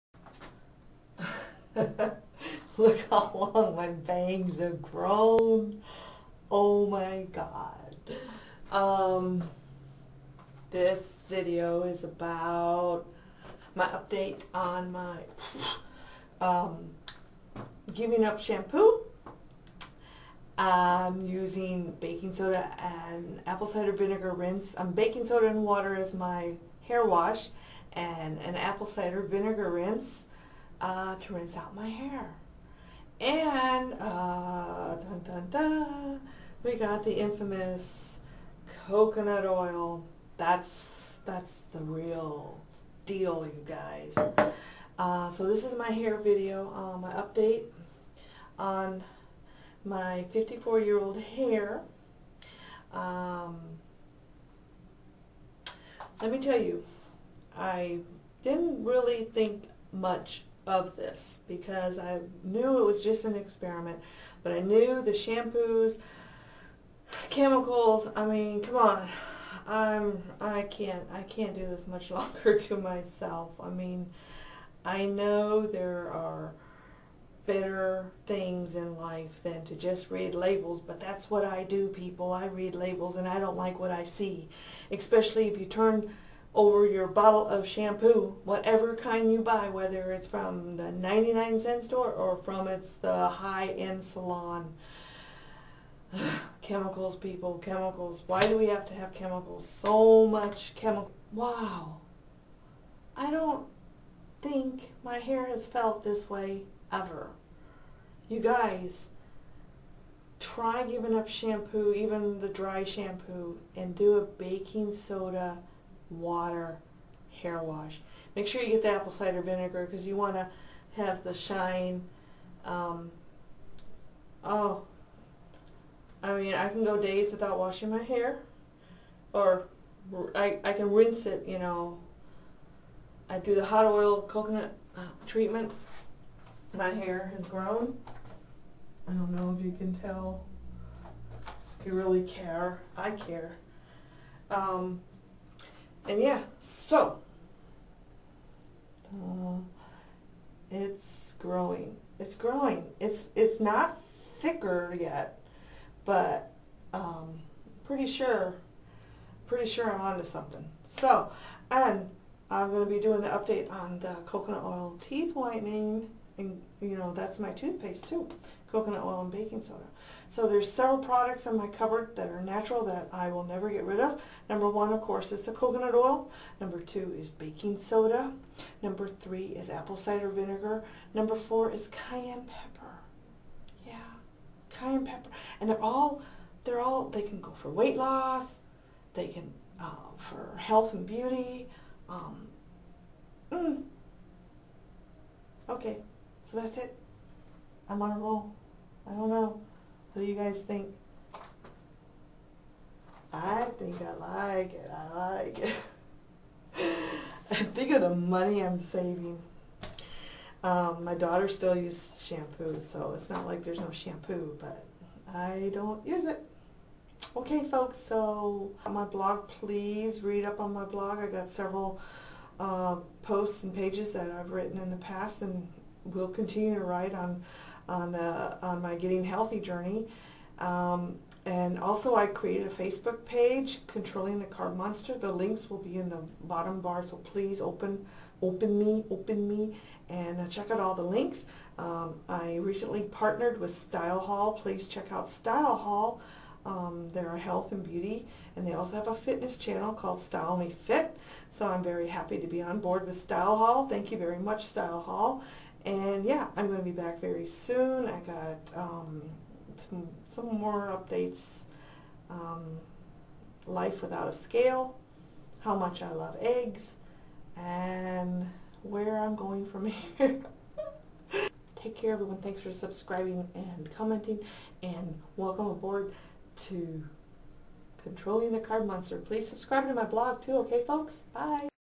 ♥StyleHaul Partner♥RESULTS!-Coconut Oil/Baking Soda-ACV Hair-Wash
I am a StyleHaul Partner!-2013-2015 This is an update on Baking Soda Hair wash..the reason why I am changing to Baking Soda hair wash with apple cider vinegar rinse and coconut oil deep treatment is shampoos and conditioners often contain synthetic silicones and silica that coat your hair, to make it "feel" nice, but leave a residue in your hair.  Synthetic silicones are derived from polymers which are soft, plastic, gel-like substances that are hard to remove from your hair and scalp.
♥Read My Blog-Controlling The Carb Monster